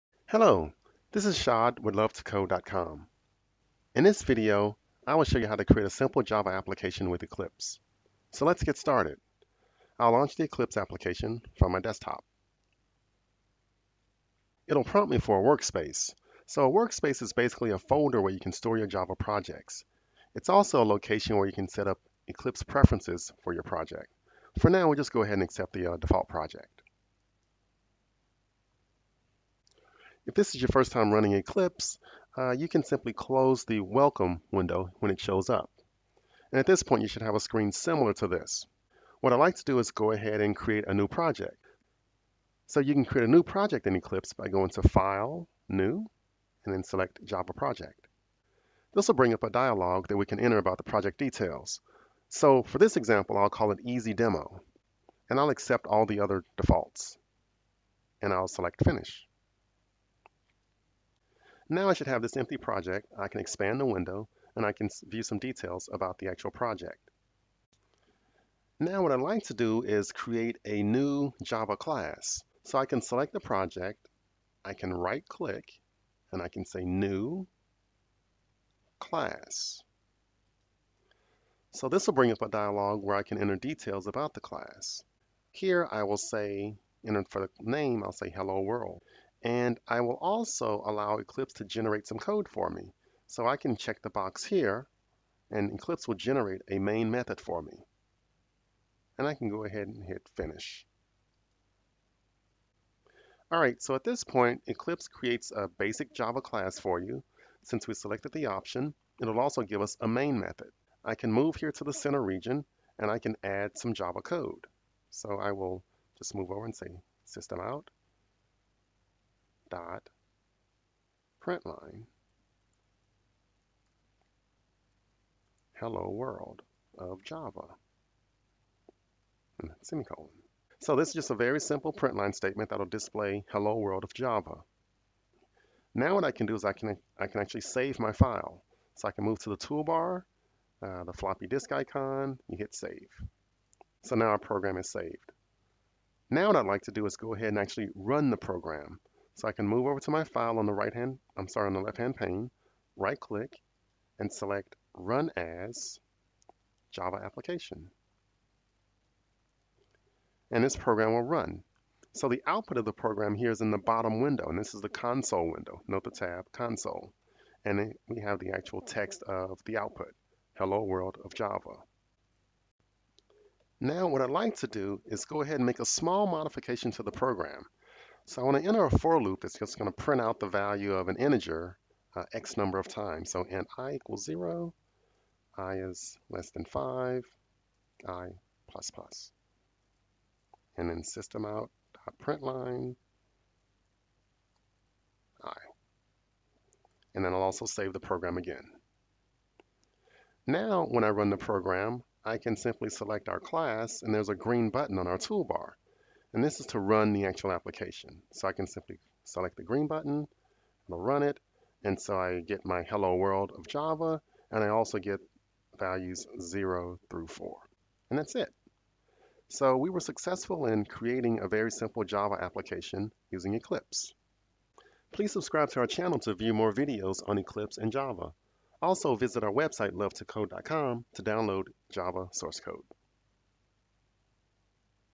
Java Eclipse Tutorial - Part 2: Create a Java Application with Eclipse
In this video tutorial, I will show you how to create a Java application in the Eclipse IDE. I'll discuss the following topics:

- Create new project
- Adding a new Java class
- Running the application

Video Transcript
Hello. This is Chad (shod) with luv2code.com. In this video, I will show you how to create a simple Java application with Eclipse. Let's get started.

Time - 00:09
I'll launch the Eclipse application from my desktop. It'll prompt me for a workspace. A workspace is basically a folder where you can store your Java projects. It's also a location where you can set up Eclipse preferences for your project. For now, we'll just go ahead and accept the default project.

Time - 00:29
If this is your first time running Eclipse, you can simply close the welcome window when it shows up. At this point, you should have a screen similar to this. What I'd like to do is go ahead and create a new project. You can create a new project in Eclipse by going to File, New, and then select Java Project. This'll bring up a dialog that we can enter about the project details. For this example, I'll call it "ezdemo" and I'll accept all the other defaults, and I'll select Finish.

Time - 01:00
Now I should have this empty project. I can expand the window and I can view some details about the actual project. Now, what I'd like to do is create a new Java class. So I can select the project, I can right-click, and I can say New, Class. This'll bring up a dialog where I can enter details about the class. Here I will say, entered for the name, I'll say, "HelloWorld" and I will also allow Eclipse to generate some code for me, so I can check the box here and Eclipse will generate a main method for me.

Time - 01:42
All right. At this point, Eclipse creates a basic Java class for you. Since we selected the option, it'll also give us a main method. I can move here to the center region and I can add some Java code. I will just move over and say, System.out.println - Hello World of Java. This is just a very simple println statement that'll display "Hello World of Java". Now what I can do is I can actually save my file. I can move to the toolbar, the floppy disk icon, and hit save. Now our program is saved.

Time - 02:26
Now what I'd like to do is go ahead and actually run the program. I can move over to my file on the right hand, sorry, on the left hand pane, right-click, and select Run As, Java Application. This program will run. The output of the program here is in the bottom window. This is the console window. Note the tab Console. In it, we have the actual text of the output. Hello World of Java.

Time - 02:55
Now what I'd like to do is go ahead and make a small modification to the program. When I enter a for loop, it's just going to print out the value of an integer x number of times. So "int i=0 i less than 5; i++" and then “System.out.println - i” and then I'll also save the program again. Now when I run the program, I can simply select our class, and there's a green button on our toolbar. This is to run the actual application. I can simply select the green button, it'll run it, and so I get my "Hello World of Java" and I also get values 0 through 4. That's it.

Time - 03:39
We were successful in creating a very simple Java application using Eclipse.